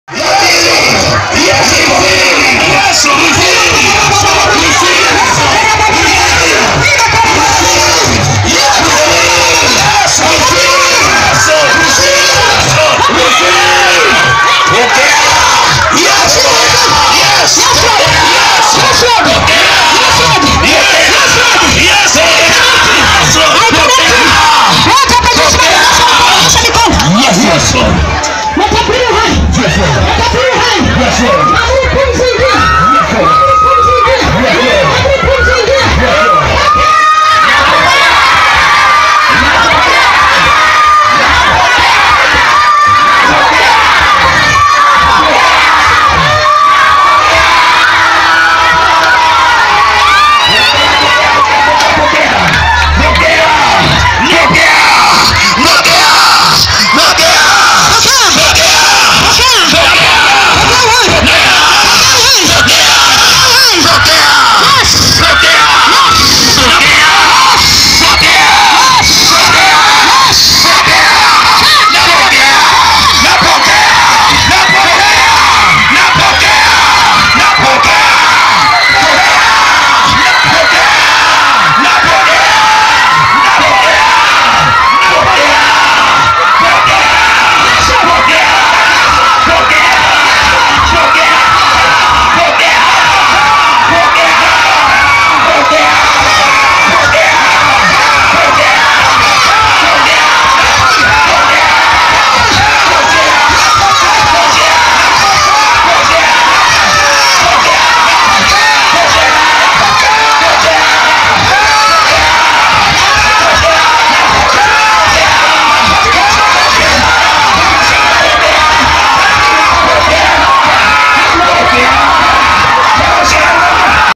05/11/2016 Saturday evening service
Impartation of new generation spirit
By prophetess ESTHER BUKUKU PMIC Moshi